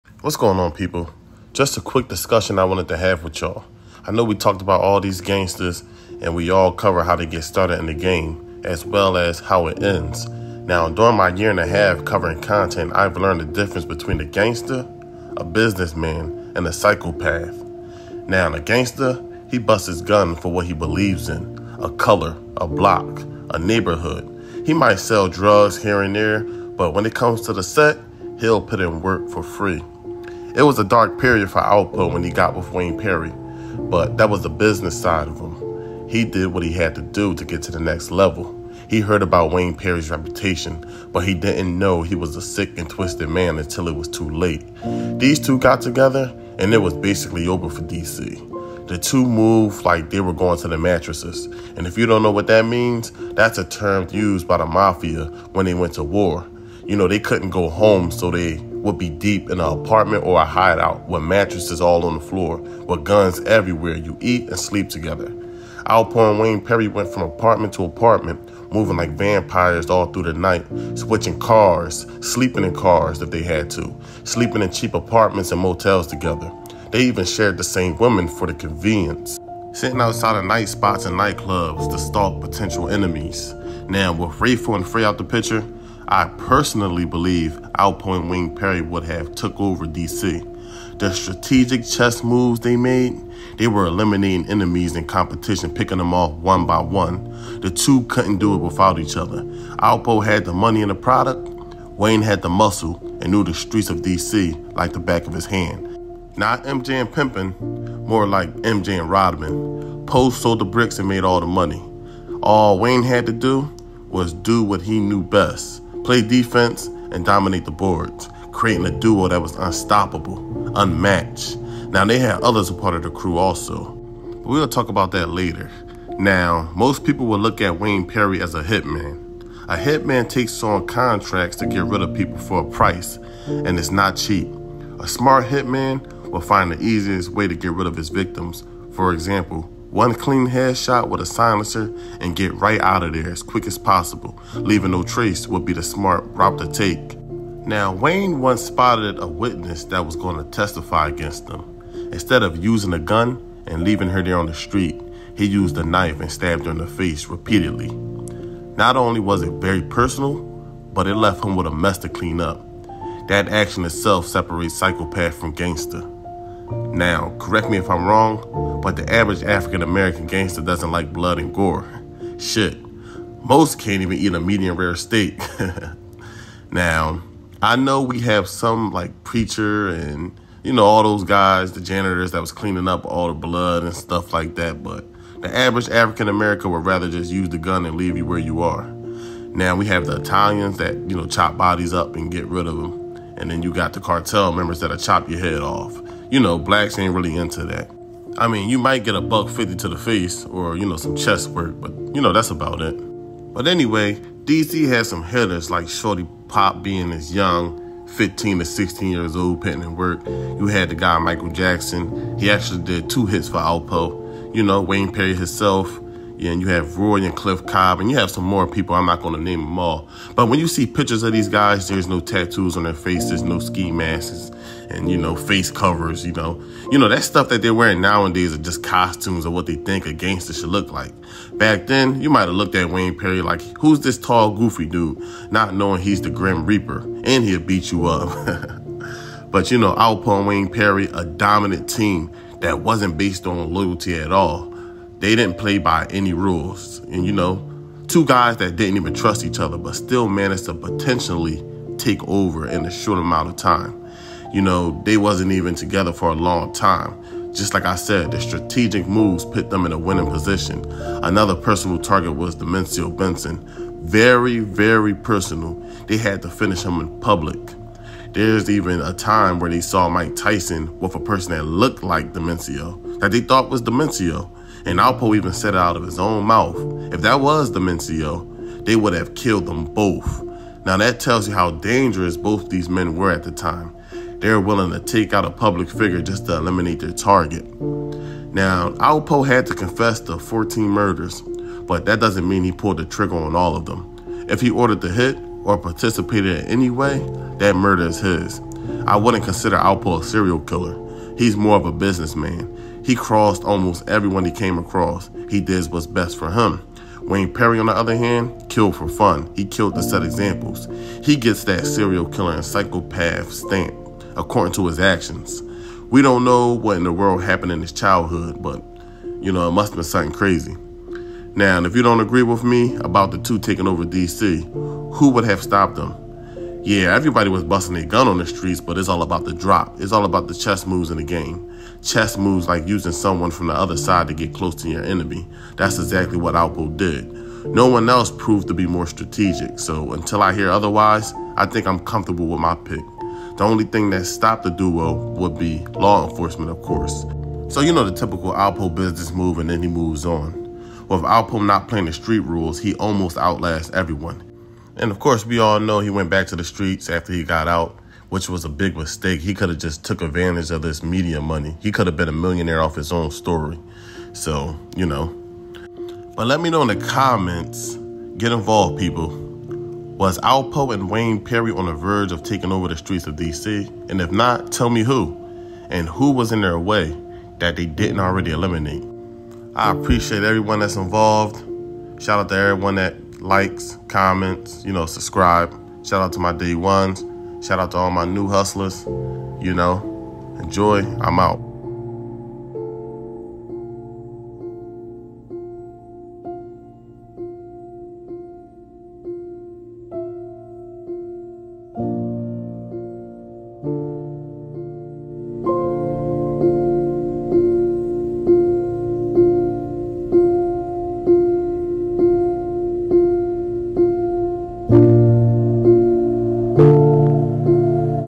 Dominate Duo - Alpo & Wayne Perry
Did Alpo & Silk almost take over the streets of DC ? Tune in